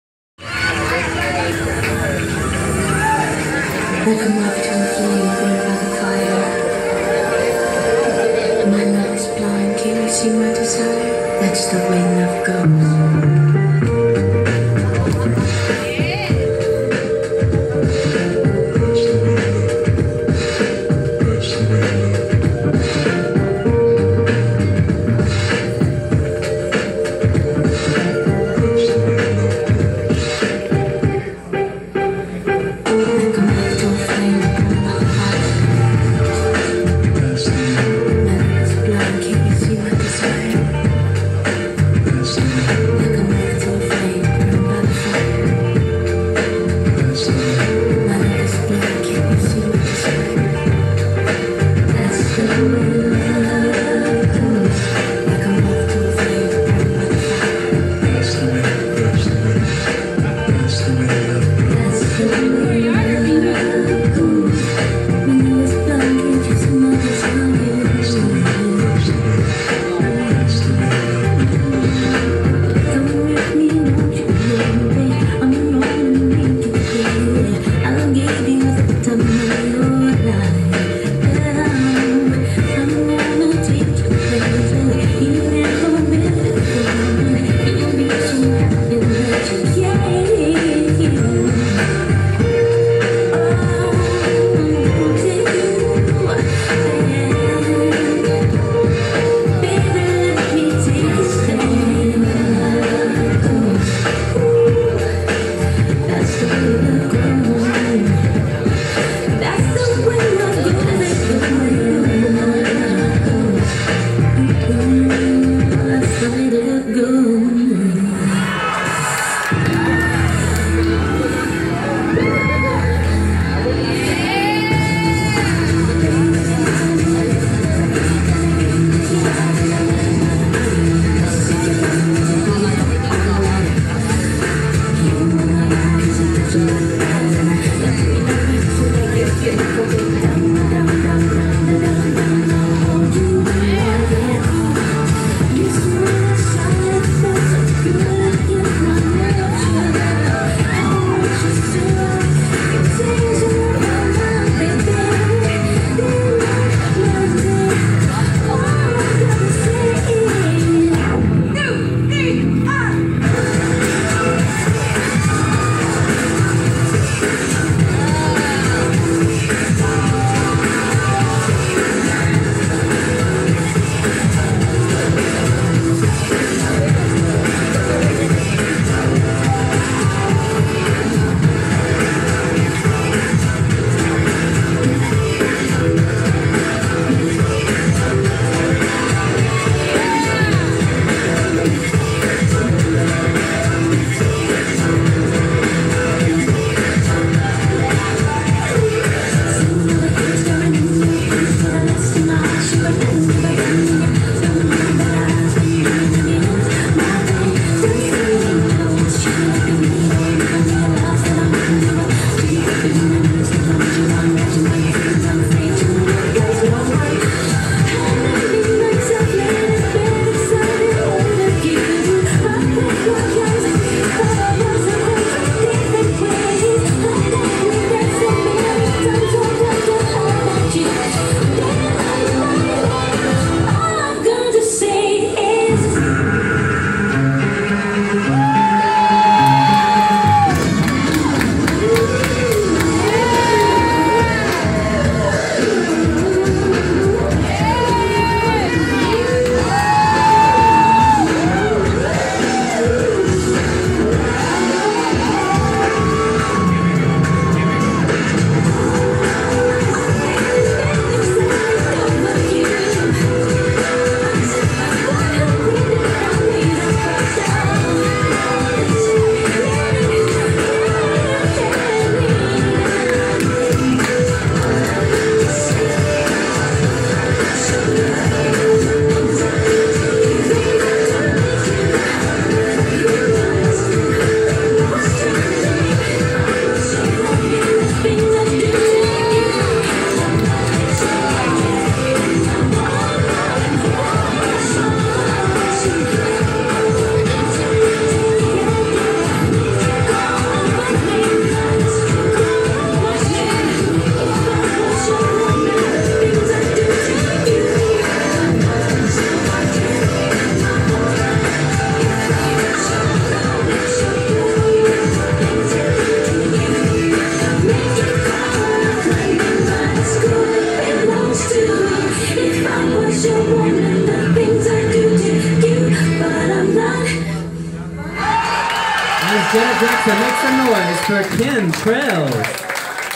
Dessie's Drag Race All Grown Up, Illusion Challenge, K'hem Trailz
K'hem Trailz as Janet Jackson at Dessie's Drag Race All Grown Up, Illusion Challenge, at Michael's Outpost